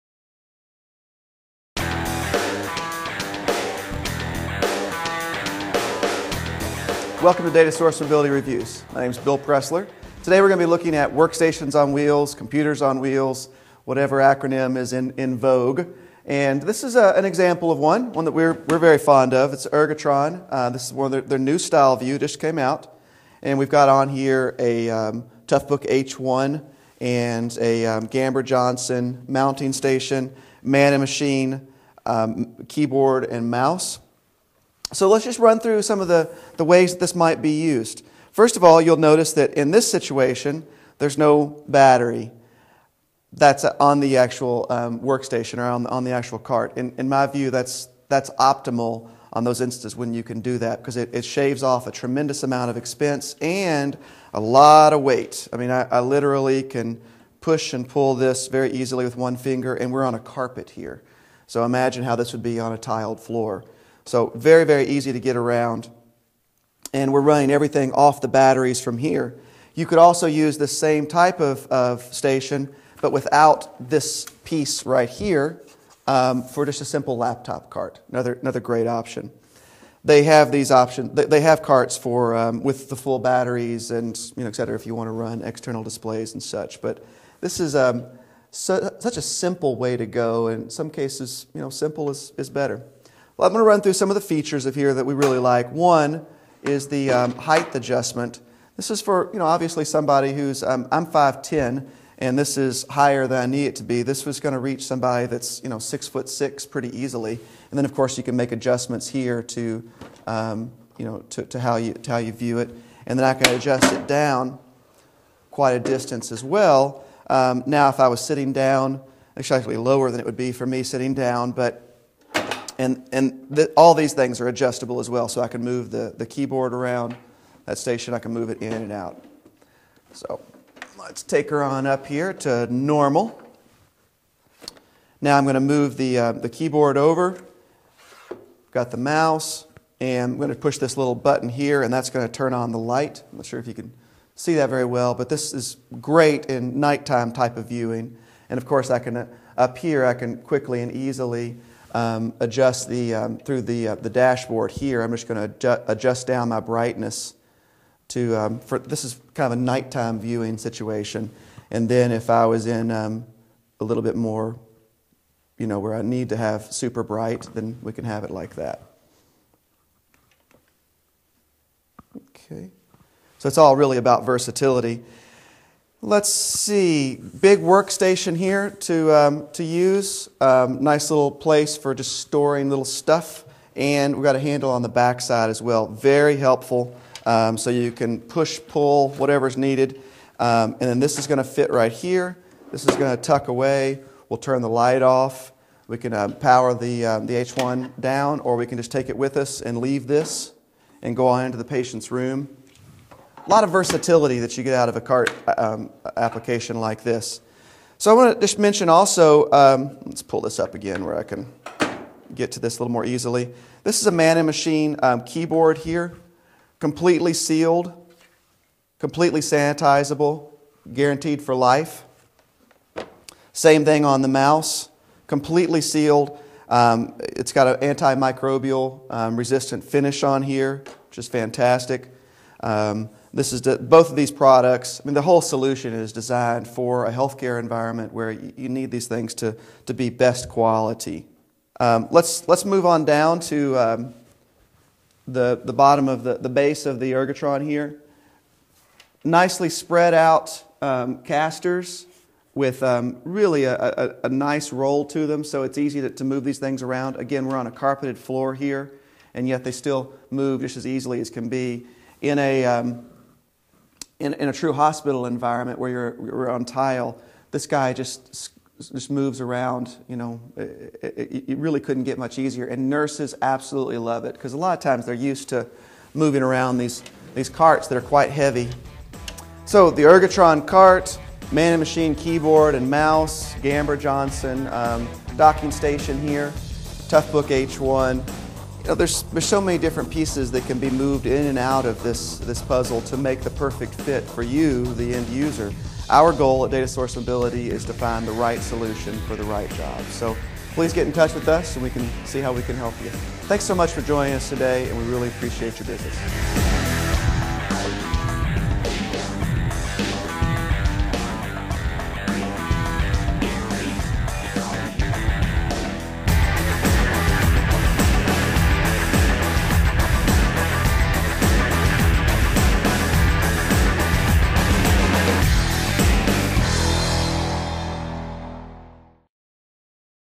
Ergotron Cart, Man & Machine Keyboard & Mouse Review
The Ergotron StyleView cart provides customers with a flexible, lightweight mobile workstation for hospitals. And with the Man & Machine keyboard and mouse, coupled with the Panasonic Toughbook H1 Health, this is the ideal solution for clinicians.